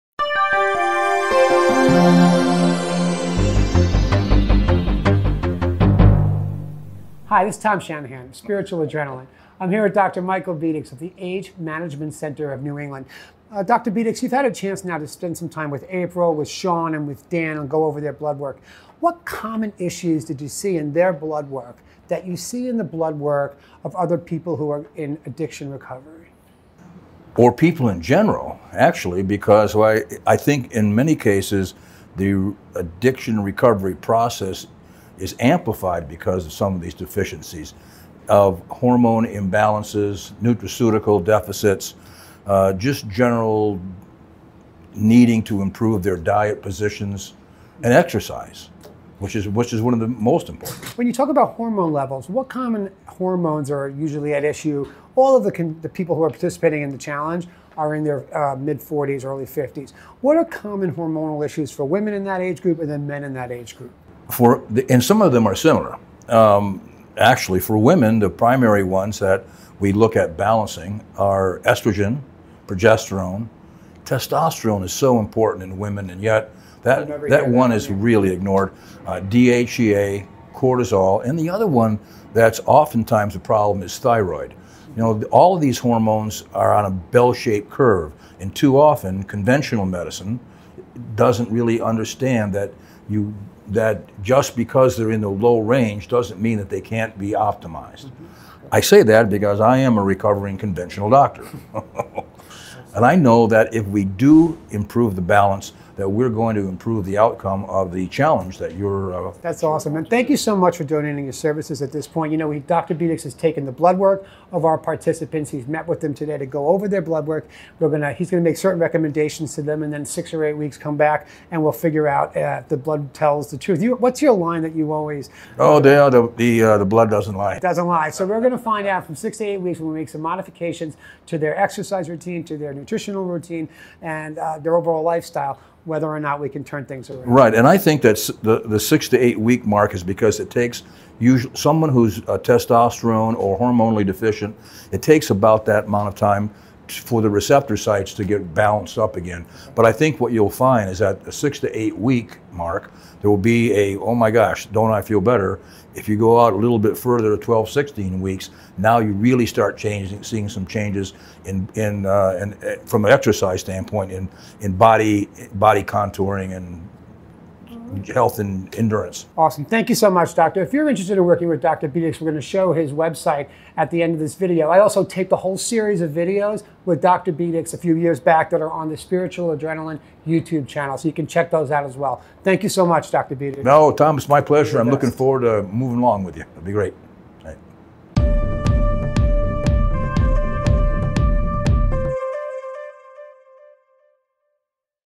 The SA Challenge: The Role of Hormones and Bloodwork.
Tom interviews Dr. Michael Bedecs, a D.O. and Hormone Replacement Specialist about why its important to test your blood when getting started on a program of lifestyle modification.